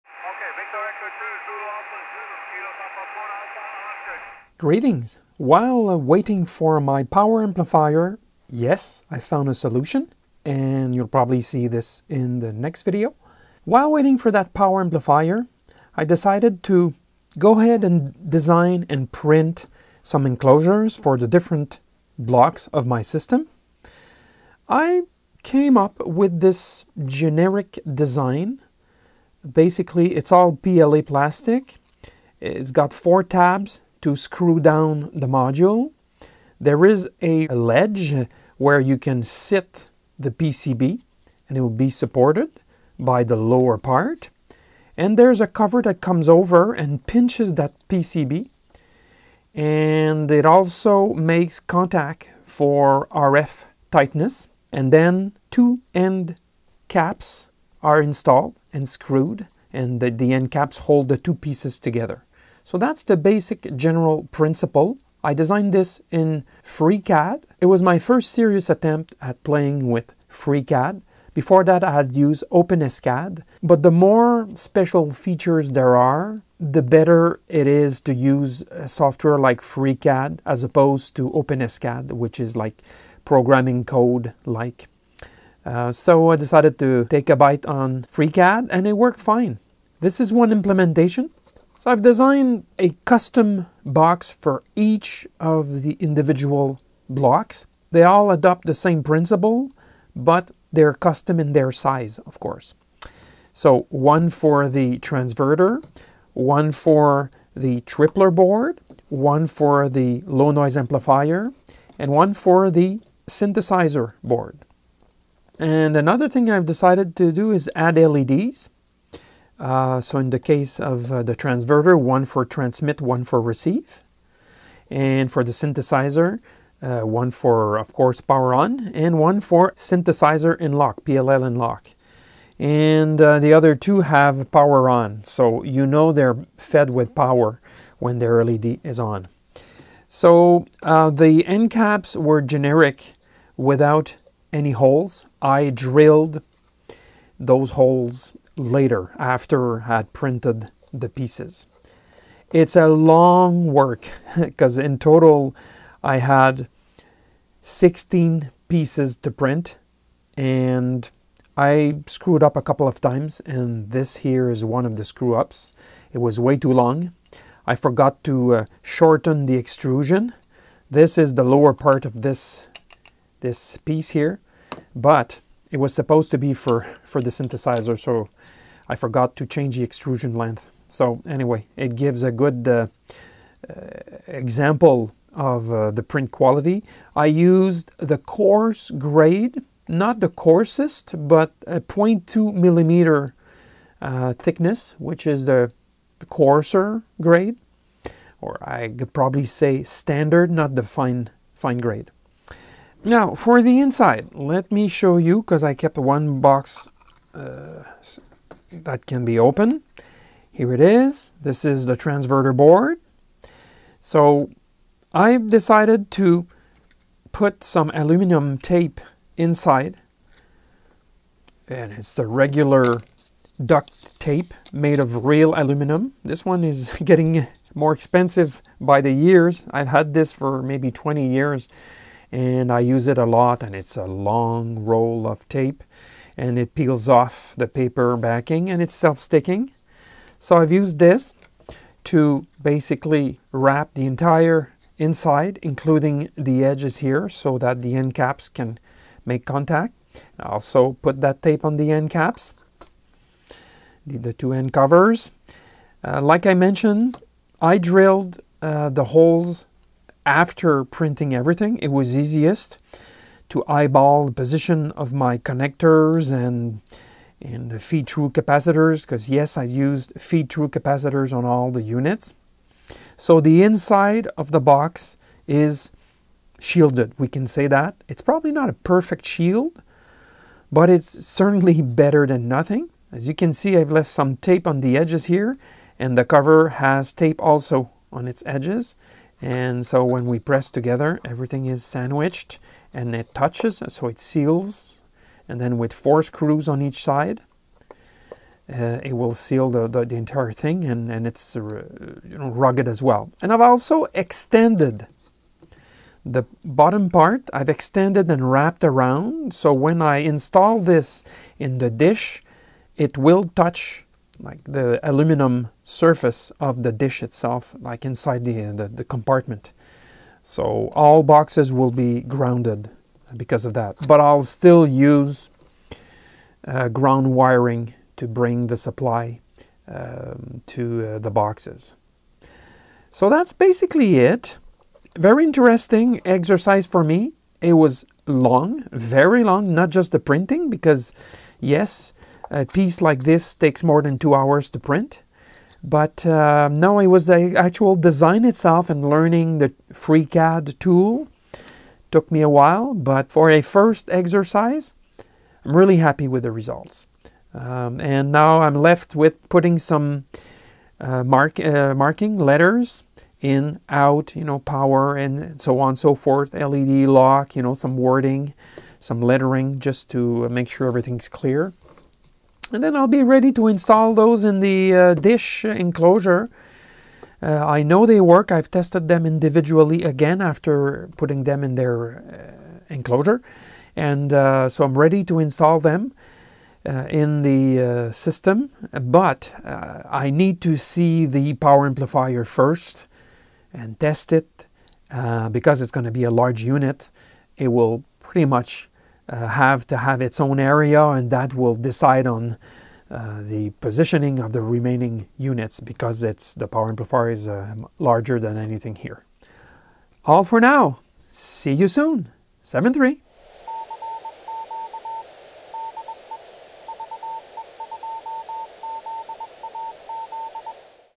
10 GHz System, Part 14: 3D-Printed Enclosures...Why not?
Since all the PCBs that make the 10G system have custom shapes, I have decided to design and 3D-print the enclosures. Here is what I did. And by-the-way, they are RF-tight!